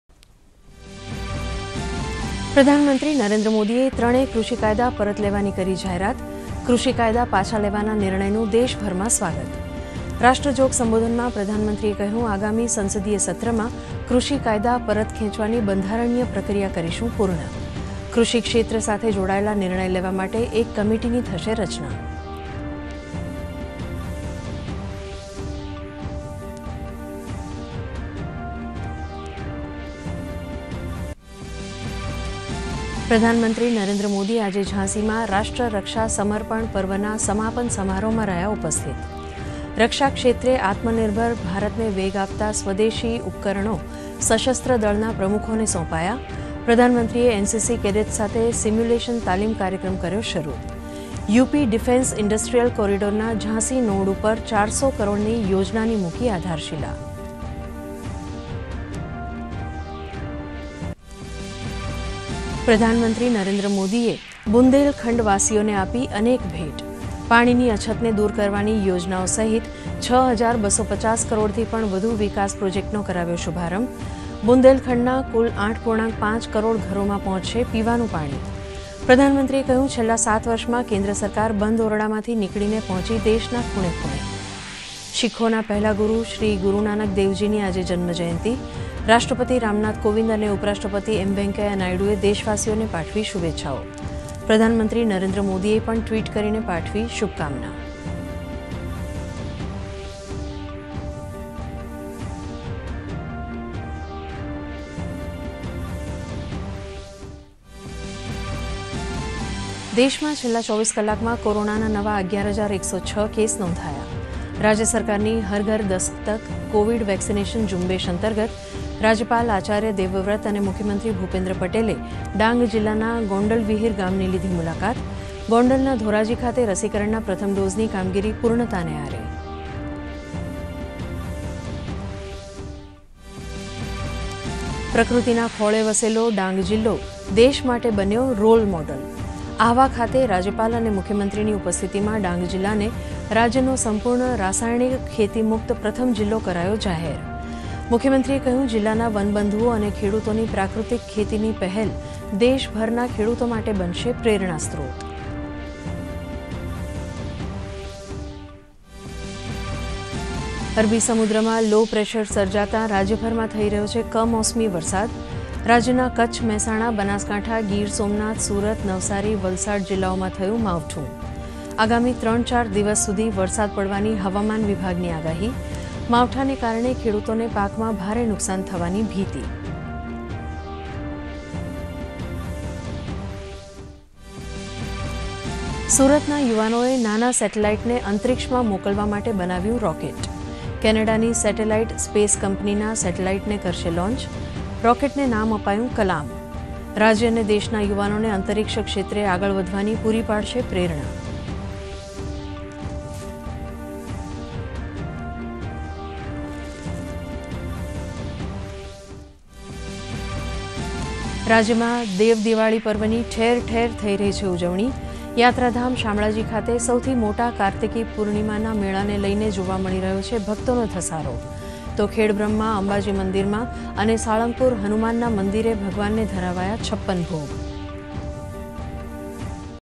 પ્રધાનમંત્રીએ NCC કેડેટ્સ માટે સિમ્યુલેશન તાલીમ કરી શરૂ અને અન્ય મહત્વના સમાચાર | Headlines @7:00PM
1.. પ્રધાનમંત્રી નરેન્દ્ર મોદીએ ત્રણેય કૃષિ કાયદા પરત લેવાની કરી જાહેરાત...  કૃષિ કાયદા પાછા લેવાના નિર્ણયનું દેશભરમાં સ્વાગત... રાષ્ટ્રજોગ સંબોધનમાં પ્રધાનમંત્રીએ કહ્યું, આગામી સંસદીય સત્રમાં,,  કૃષિ કાયદા પરત ખેંચવાની બંધારણીય પ્રક્રિયા કરીશું પૂર્ણ.. કૃષિ ક્ષેત્ર સાથે જોડાયેલા નિર્ણયો લેવા માટે ,,  એક કમિટીની થશે રચના..

2. પ્રધાનમંત્રી નરેન્દ્ર મોદી આજે ઝાંસીમાં ,, રાષ્ટ્ર રક્ષા સમર્પણ પર્વના સમાપન સમારોહમાં રહ્યા ઉપસ્થિત... રક્ષા ક્ષેત્રે આત્મનિર્ભર ભારતને વેગ આપતા સ્વદેશી રૂપે બનેલા ઉપકરણો સશસ્ત્રદળના પ્રમુખોને સોંપ્યા... પ્રધાનમંત્રીએ NCC કેડેટ્સ માટે સિમ્યુલેશન તાલીમ કાર્યક્રમ કર્યો શરૂ. યુપી ડિફેન્સ ઇન્ડસ્ટ્રીયલ કોરિડોરના ઝાંસી નોડ ઉપર 400 કરોડની યોજનાની મુકી આધારશીલા .

3. પ્રધાનમંત્રી નરેન્દ્ર મોદીએ બુંદેલખંડવાસીઓને આપી અનેક ભેટ.. પાણીની અછતને દૂર કરવાની યોજનાઓ સહિત 6 હજાર 250 કરોડથી પણ વધુ વિકાસ પ્રોજેક્ટનો કરાવ્યો શુભારંભ. બુંદેલખંડનાં કુલ 8.5 કરોડ ઘરોમાં પહોંચશે પીવાનું પાણી. પ્રધાનમંત્રીએ કહ્યું... છેલ્લા 7 વર્ષમાં કેન્દ્ર સરકાર બંધ ઓરડા માંથી નીકળી પહોંચી દેશનાં ખૂણે ખૂણે...

4. શીખોના પહેલા ગુરુ શ્રી ગુરુનાનક દેવજીની આજે જન્મ જયંતિ.. રાષ્ટ્રપતિ રામનાથ કોવિંદ અને ઉપરાષ્ટ્રપતિ એમ વેંકૈયા નાયડુએ દેશવાસીઓને પાઠવી શુભેચ્છા. પ્રધાનમંત્રી નરેન્દ્ર મોદીએ ટ્વીટ કરી પાઠવી શુભકામના...

5. દેશમાં છેલ્લા 24 કલાકમાં કોરોનાના નવા 11 હજાર 106 કેસ નોંધાયા. રાજ્ય સરકારની ‘હરઘર દસ્તક’ કોવિડ વેક્સીનેસન ઝુંબેશ અંતર્ગત રાજયપાલ આચાર્ય દેવવ્રત અને મુખ્યમંત્રી ભૂપેન્દ્ર પટેલે ડાંગ જિલ્લાના ગોંડલવિહિર ગામની લીધી મુલાકાત. તો ગોંડલનાં ધોરાજી ખાતે રસીકરણનાં પ્રથમ ડોઝની કામગીરી પુર્ણતાને આરે...

6.. પ્રકૃતિના ખોળામાં વસેલો ડાંગ જિલ્લો દેશ માટે બન્યો રોલ મોડેલ. આહવા ખાતે રાજ્યપાલ અને મુખ્યમંત્રીની ઉપસ્થિતિમાં ડાંગ જિલ્લાને રાજ્યનો સંપૂર્ણ રાસાયણિક ખેતીમુક્ત પ્રથમ જિલ્લો કરાયો જાહેર. મુખ્યમંત્રીએ કહ્યુ, જિલ્લાના વનબંધુઓ અને ખેડૂતોની પ્રાકૃતિક ખેતીની પહેલ દેશભરના ખેડૂતો માટે બનશે પ્રેરણાસ્ત્રોત.

7.. અરબી સમુદ્રમાં લો પ્રેશર સર્જાતાં રાજ્યમાં થઈ રહ્યો છે કમોસમી વરસાદ. રાજ્યનાં કચ્છ, મહેસાણા, બનાસકાંઠા, ગીર સોમનાથ, સુરત, નવસારી, વલસાડ જિલ્લાઓમાં થયું માવઠું ... આગામી ત્રણ - ચાર દિવસ સુધી વરસાદ પડવાની હવામાન વિભાગની આગાહી... માવઠાને કારણે ખેડૂતોને પાકમાં ભારે નુકસાન થવાની ભીતી...

8...સુરતનાં યુવાનોએ નાના સેટેલાઈટને અંતરીક્ષમાં મોકલવા માટે બનાવ્યું રોકેટ. કેનેડાની સેટેલાઈટ સ્પેસ કંપનીનાં સેટેલાઈટને કરશે લોન્ચ. રોકેટને નામ આપ્યું કલામ. રાજ્ય અને દેશનાં યુવાનોને અંતરીક્ષ ક્ષેત્રે આગળ વધવાની પુરી પાડશે પ્રેરણા...

9. રાજ્યમાં દેવ દિવાળી પર્વની ઠેર ઠેર થઈ રહી છે ઉજવણી. યાત્રાધામ શામળાજી ખાતે સૌથી મોટા કાર્તિકી પૂનમના મેળાને લઇને જોવા મળી રહ્યો છે ભક્તોનો ધસારો. તો ખેડબ્રહ્મા અંબાજી મંદિરમાં અને સાળગંપુર હનુમાનના મંદિરે ભગવાનને ધરાવાયા છપ્પન ભોગ...